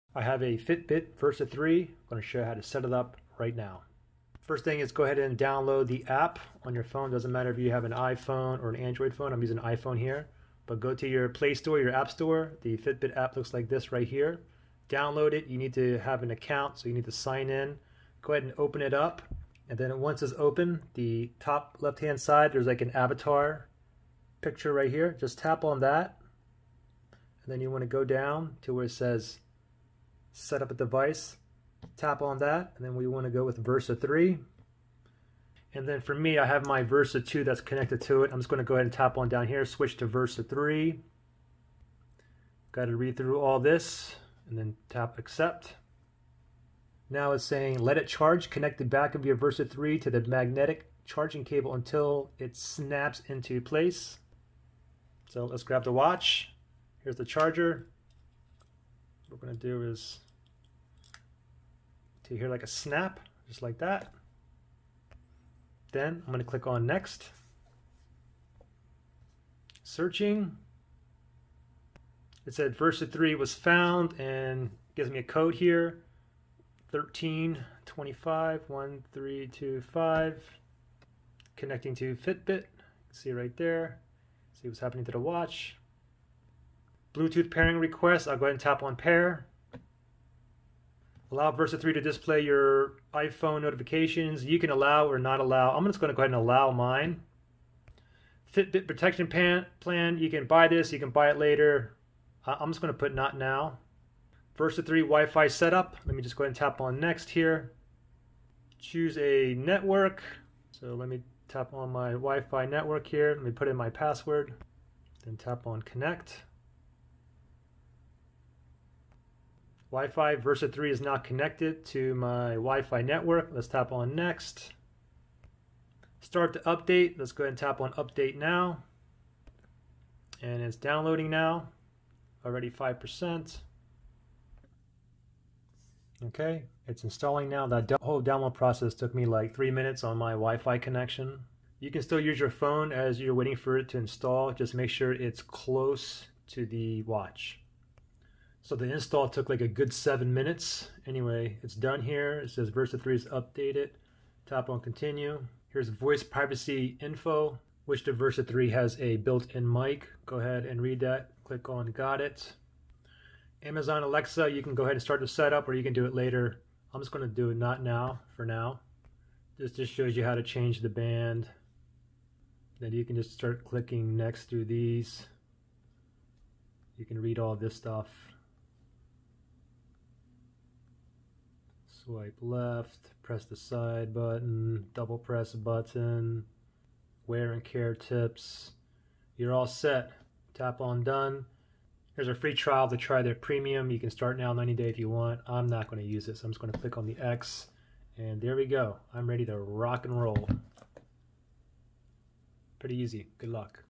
Fitbit Versa 3: How to Setup (Pair/Sync/Connect to Phone)
In video tutorial I show you how to setup (pair/sync/connect) your Fitbit Versa 3 watch to your phone (iPhone or Android) step by step.  Hope this helps.

Fitbit Versa 3

Also please do at your own risk as i am not responsible should anything go wrong.